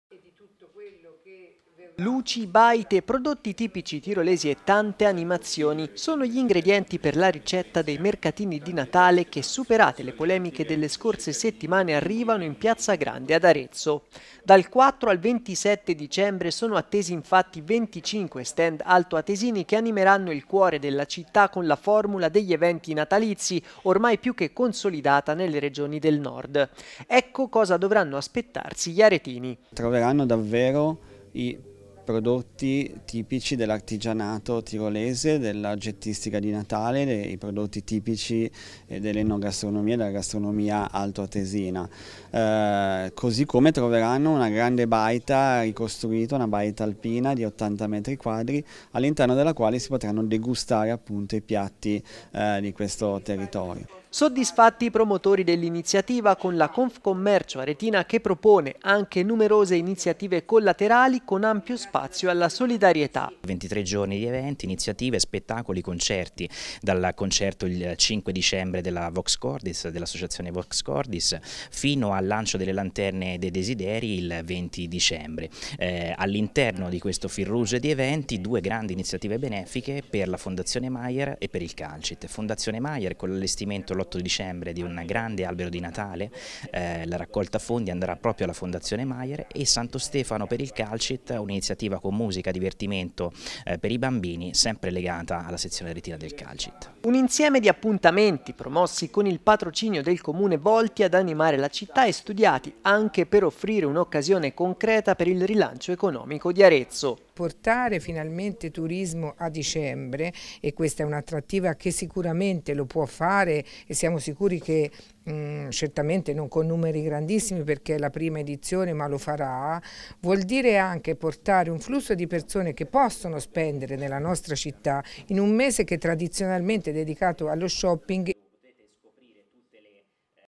Arezzo apre le porte ai Mercatini tirolesi per "Natale in piazza"
Luci, baite, prodotti tipici tirolesi e tante animazioni. Sono gli ingredienti per la ricetta dei mercatini di Natale” che, superate le polemiche delle scorse settimane arrivano in piazza Grande ad Arezzo.
Dal 4 al 27 dicembre sono attesi infatti 25 stand altoatesini che animeranno il cuore della città con la formula degli eventi natalizi, ormai più che consolidata nelle regioni del Nord.
Ecco cosa dovranno aspettarsi gli aretini.
Servizio di Riccardo Ciccarelli, riprese di Gianni Stanganini, intervista a Fabio Bongiorni - responsabile Tirol Eventi, Gianluca Rosai - Confcommercio Arezzo, Anna Lapini - pres. Confcommercio Arezzo.